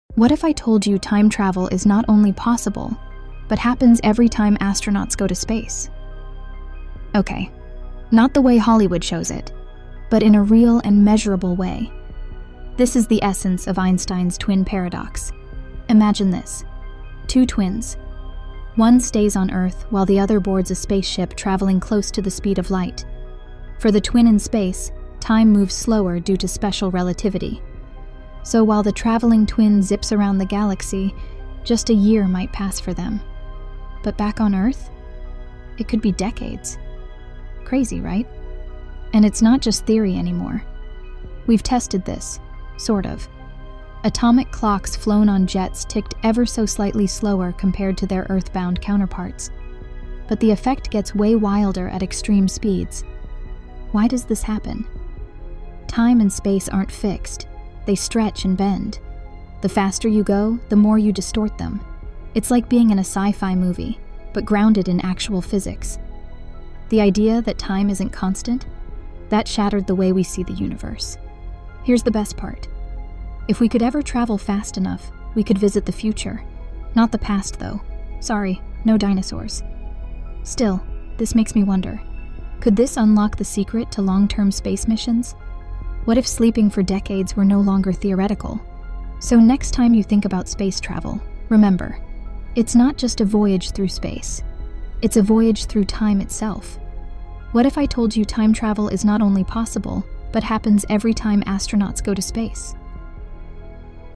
The Twin Paradox: How Space Travel MESSES With Time
Traveling through time isn’t just sci-fi—it’s built right into the fabric of physics. 🚀🕒 Quantum twist meets relativity in today’s mind-bender!